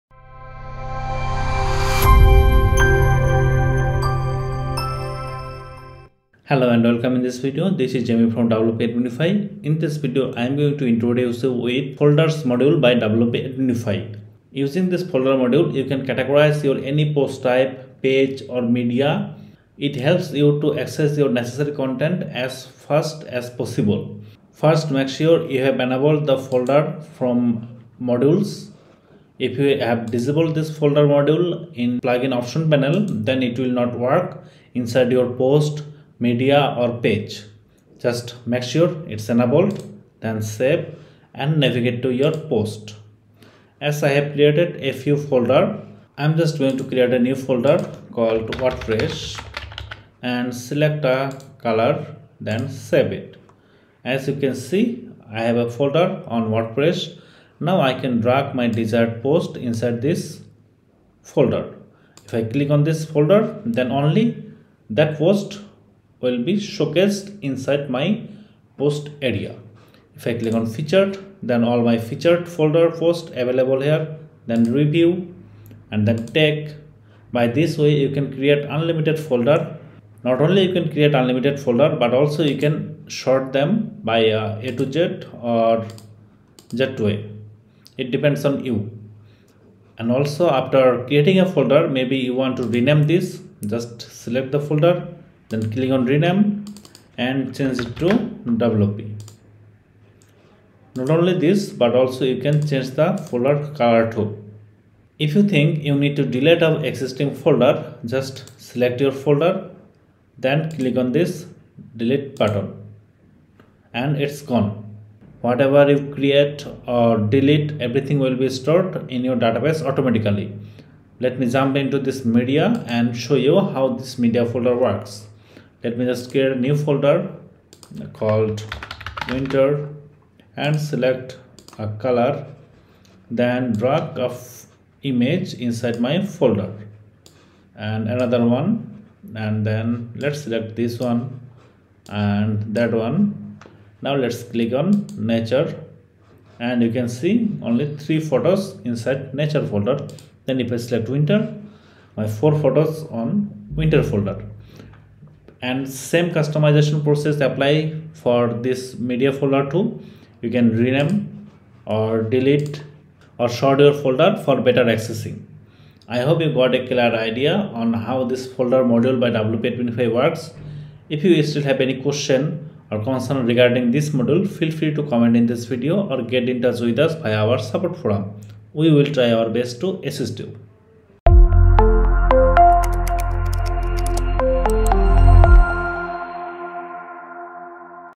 WordPress Folders For Media Library and Post Type By WP Adminify
Folders by WP Adminify is a WordPress plugin that lets you organize content in any way that you want. You can sort items into different folders and display them in a list.

👍  Organize all of your media files including images, videos, music and so on in a folder of your choice. You can also divide it by relevance to pages from the main website and posts from the blog.

👍  Web developers: you should always keep your client's website pages in order to avoid putting them into situations that can affect their work. You'll also want to mark certain folders with colour that will make it easier for them to understand what is necessary and what they can change if needed.

👍. Bloggers: Managing your own content is way easier with the right folder plugin. Give yourself an easier time by labeling your photos & content, whether by topic, category, or date.

👍  Tree View Styles: This is useful for those who have a lot of folders and files and it'll also save them a lot of time. Add as many subfolders as you need and it will look great with the Tree view.